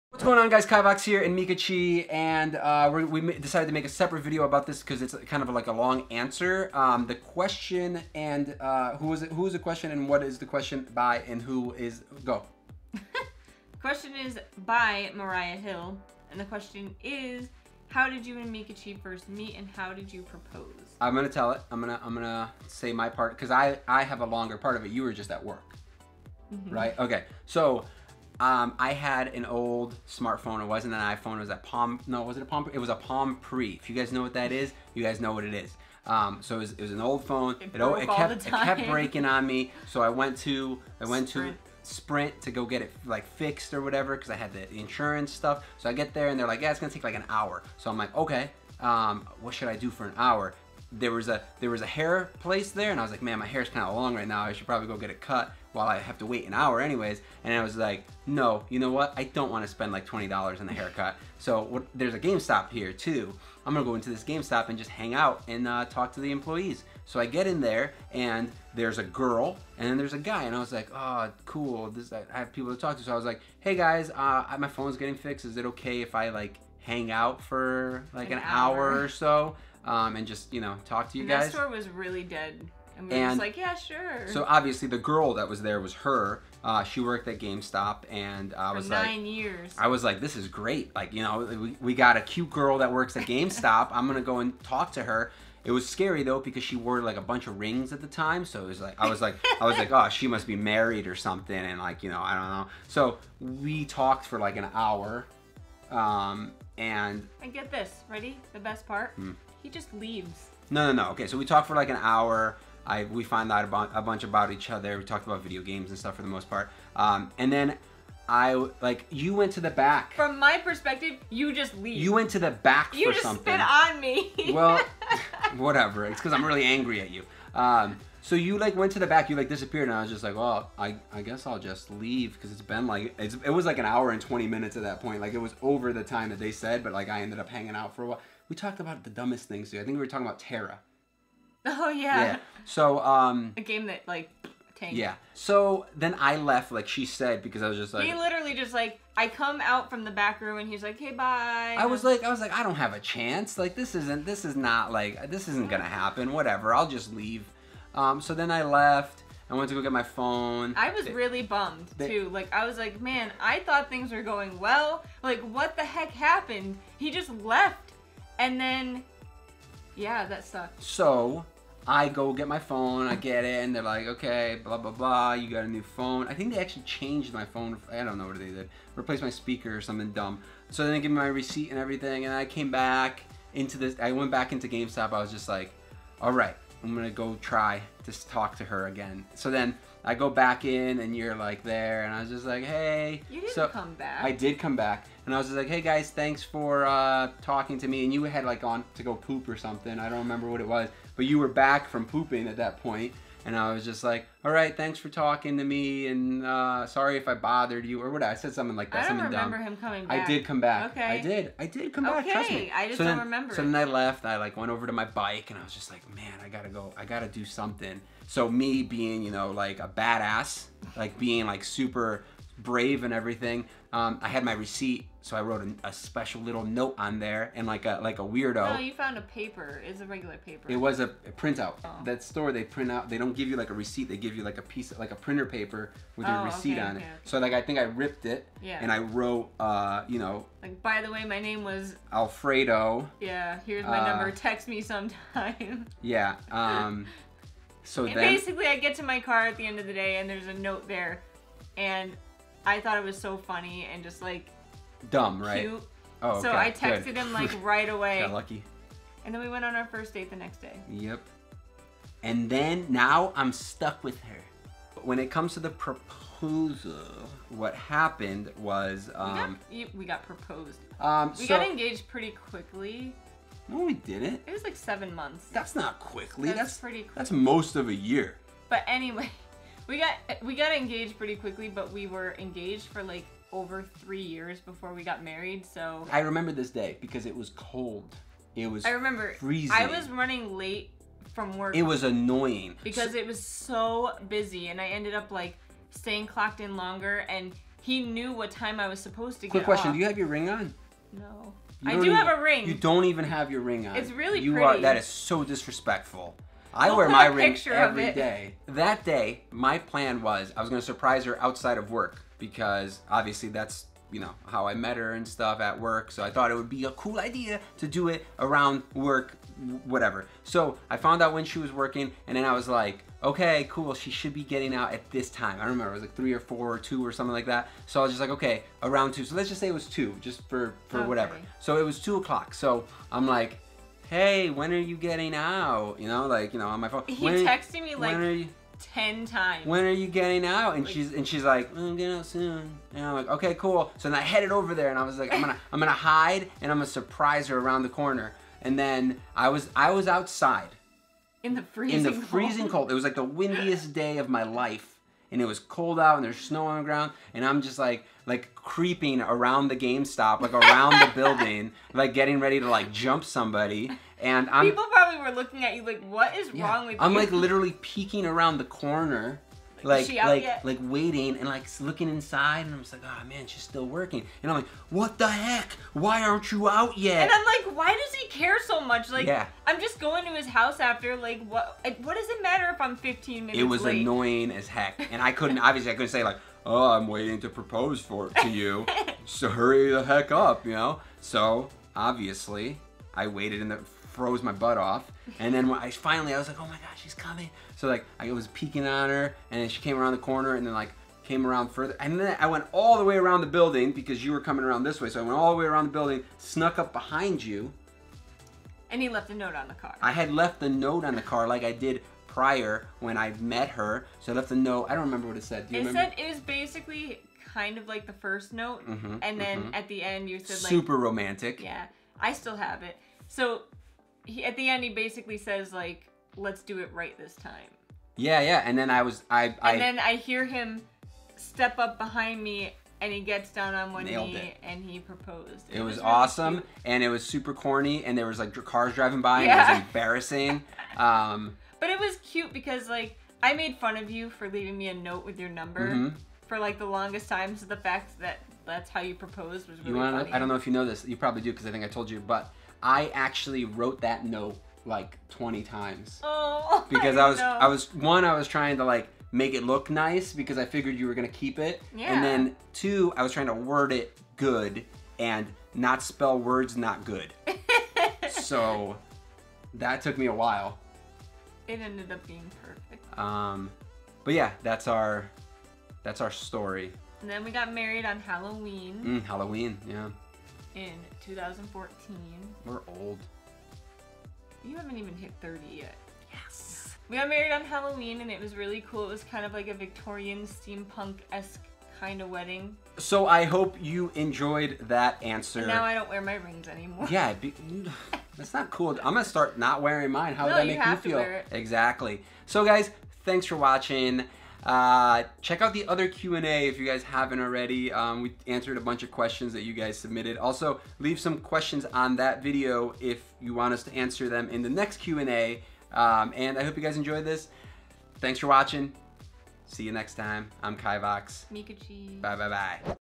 How We Met , Q & A part 2 Kivox And Meekachii
this is a really long answer that did not fit in the regular Q & A so we made a separate video on how we met for the first time, and how I finally proposed to Meekachii. and don't forget to check out part 1 of this Q and A where we answer the rest of your questions

my Gear
Lenses :
10-18mm wide angle
Laptop :
MacBook Pro